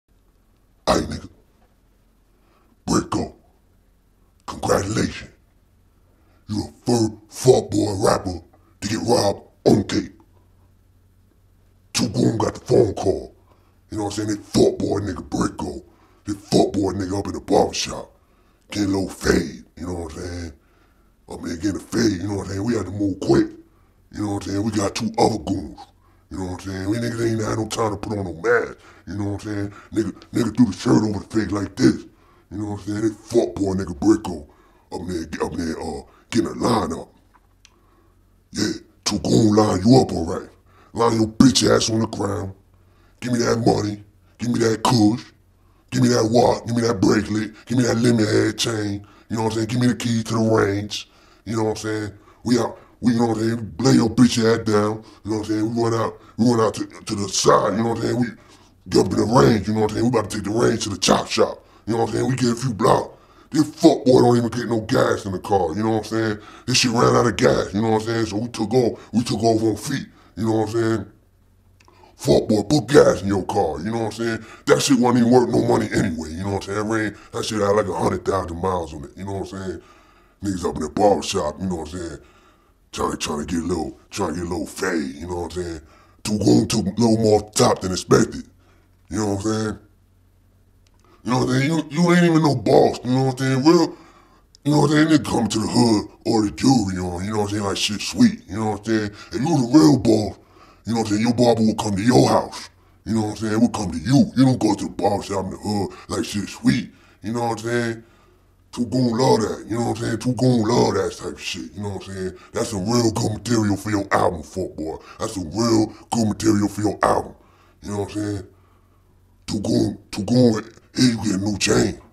2 GOONS ROB RAPPER BRISCO IN MIAMI BARBERSHOP
2 GOONS ROB CASH MONEY RECORDS RAPPER BRISCO IN MIAMI BARBERSHOP